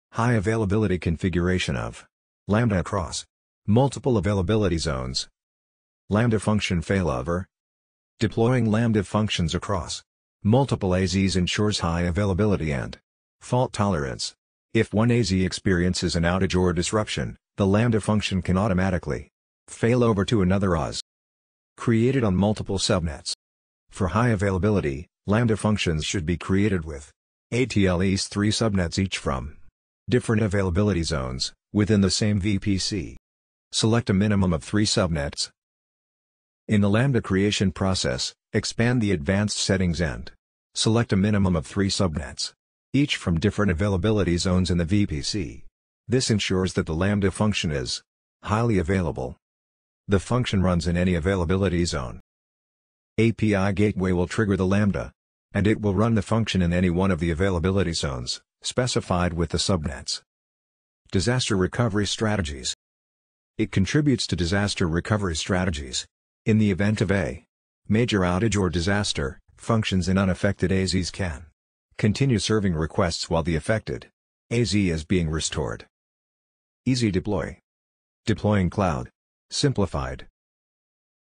Lambda Multi-Zone High Availability Configuration🚀
🚀 Deploying Lambda functions across multiple AZs ensures high availability and fault tolerance.

💪🏼 If one AZ goes down, the Lambda function seamlessly fails over to another AZ. 😍

For rock-solid high availability, create Lambda functions with a minimum of 3 subnets from different AZs within the same VPC. 🌐

During the lambda creation process, remember to expand the advanced settings and choose 3+ subnets from different AZs in the VPC.

This guarantees a highly available lambda function. ⚙️

🔗 API Gateway triggers the lambda, which runs in any specified AZ according to the chosen subnets. 💨

This configuration also strengthens disaster recovery strategies.

Even during major outages, unaffected AZs keep serving requests while the affected AZ is restored. 💡